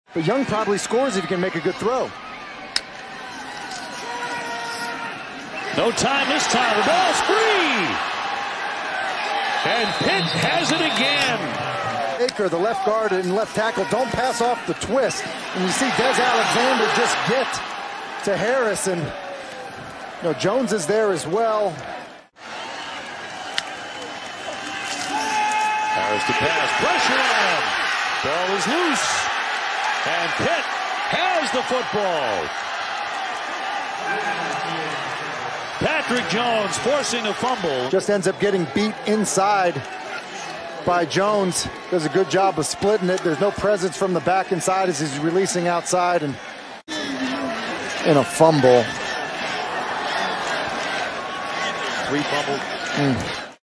Pittsburgh DL Patrick Jones II | 2019 Top Regular Season Play
Pittsburgh's Patrick Jones II is a disruptive force off the edge for the Panthers. He was a vital asset to Pitt's vaunted defensive line that made its defense one of the nation's best at sacking the quarterback. Which of Mr. Jones' plays was his finest last season? You decide right here!